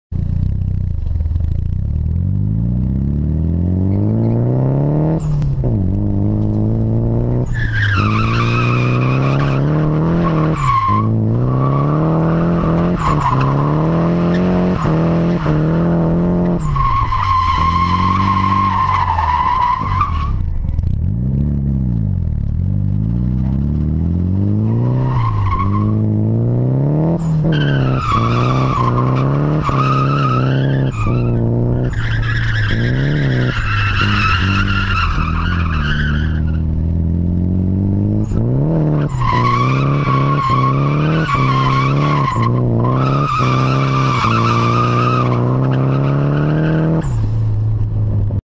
GoPro on Michaels SR20 S13 at Piedmont
more fun with the GoPro, Michael was fighting understeer in the car all day. still some tuning to be done with the suspension. that and the car could use a few extra horsepower